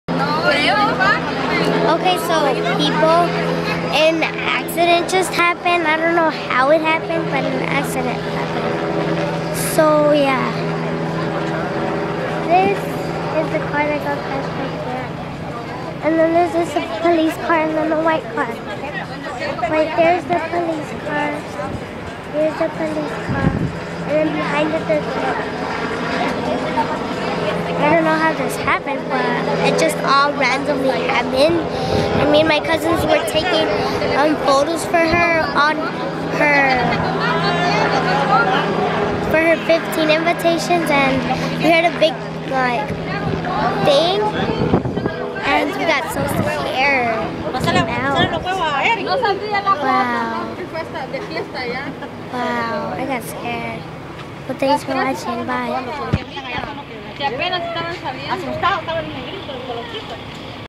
Oh Shit Biggest car crash ever my heart stopped
3 Cars hit each other omg a cop ask way happen to me I said the whole story scary man scary